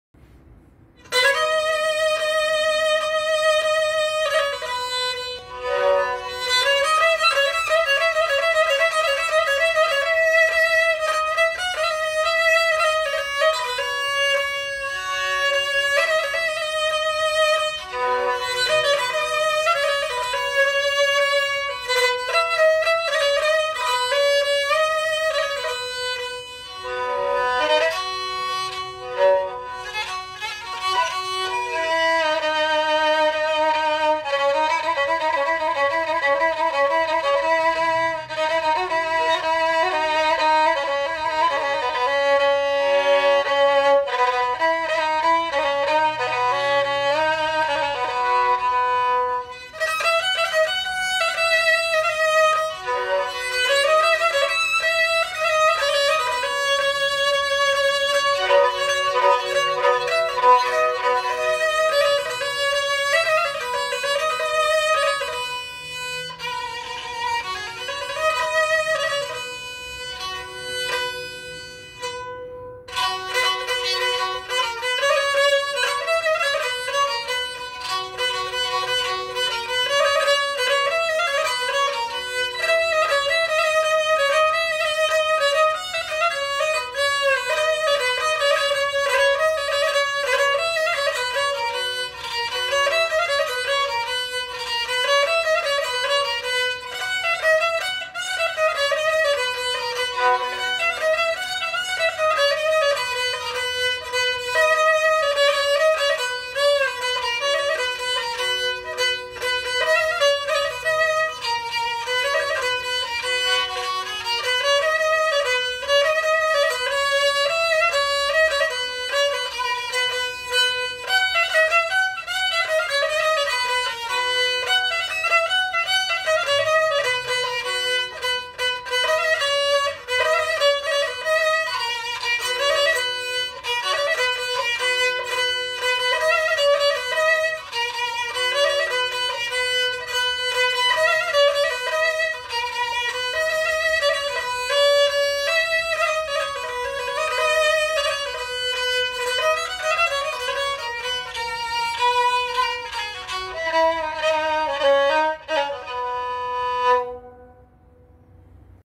کمانچه مهراب الهیاری
این ساز رو در دوره سربازی در شهر زیبای خرم آباد از استاد مراحمی عزیز خریداری کردم اوایل دهه هشتاد
من عاشق موسیقی نواحی ایران هستم مخصوصا موزیک زیبایی لری
با پوزش از نوازنده های عزیز کمانچه لری
استودیو گلها کرج
تیر ماه 1401